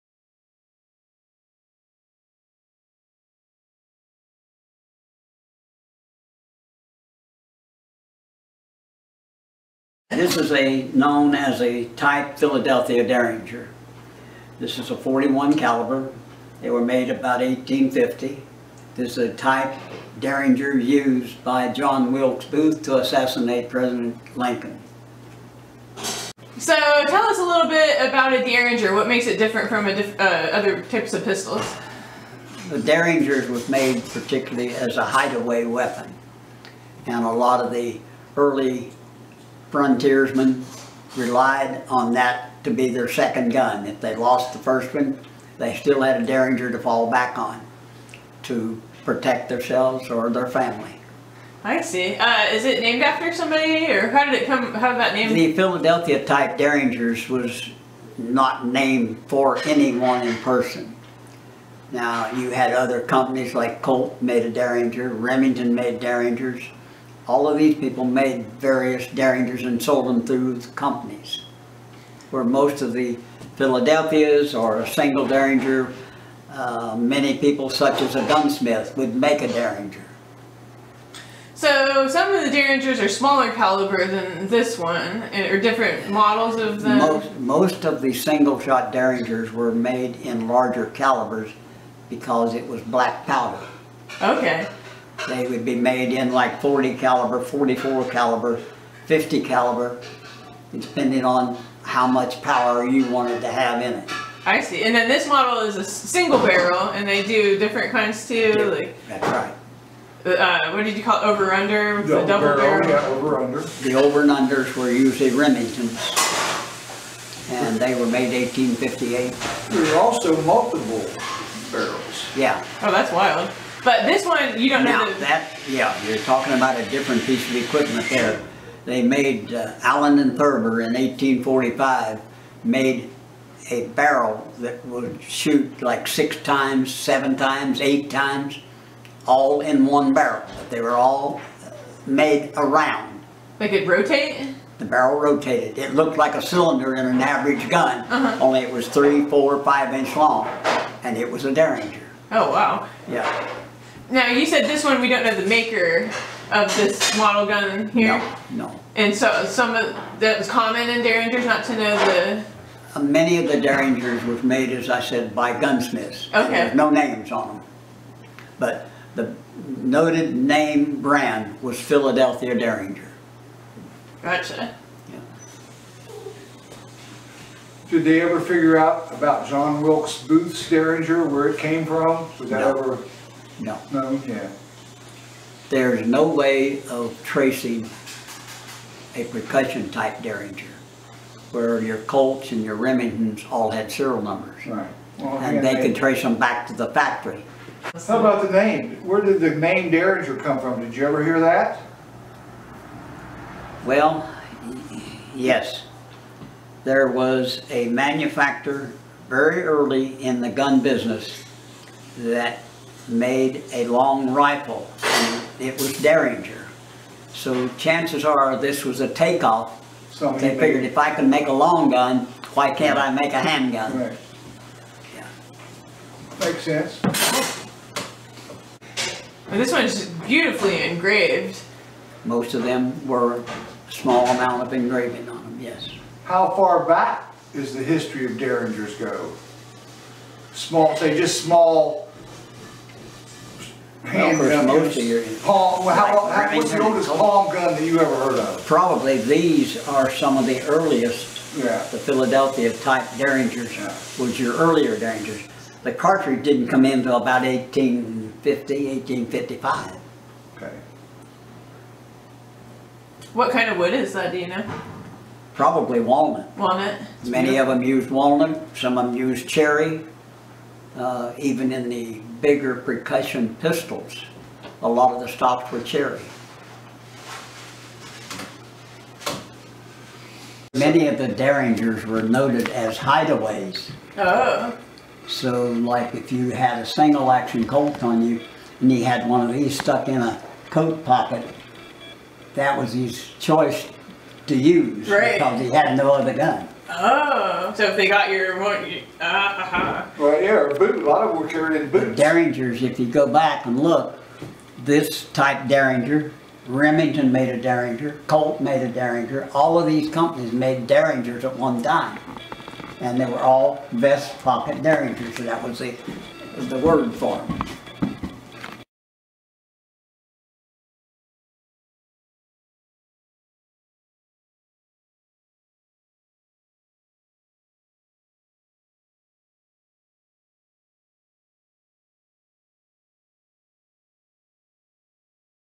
The Philadelphia Derringer: An Interview with Jack Lewis, Master Gunsmith
Jack Lewis, Master Gunsmith, shares his knowledge of the Philadelphia Derringer.

Southern Ohio Museum & Cultural Center presents, "Jack Lewis:  The Art of Making a Bang!"  This exhibition of Jack Lewis' work is on display from March 1 - April 30, 2022.

Thank you to Lacy Davis for providing the photography and cinematography for this exhibition!